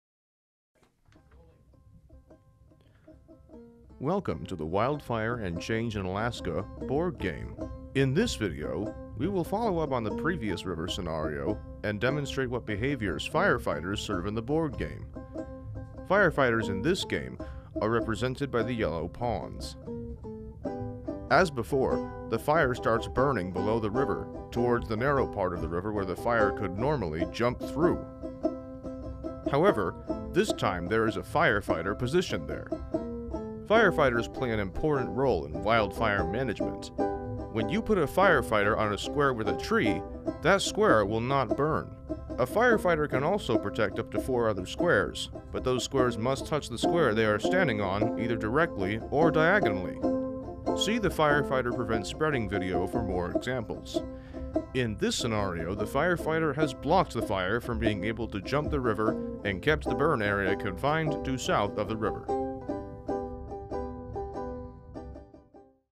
Wildfire and Change in Alaska Board Game: Part 6 - Fire fighter stops the burn at the river
Part 6 of a series of videos describing a board game which is part of a curriculum for grades 3-5 titled, "Wildfire and Change in Alaska" created by Alaska NSF EPSCoR's Fire & Ice project. The game models wildfire behavior under different scenarios. In this video, the fire approaches a river, and a fire fighter comes to our aid.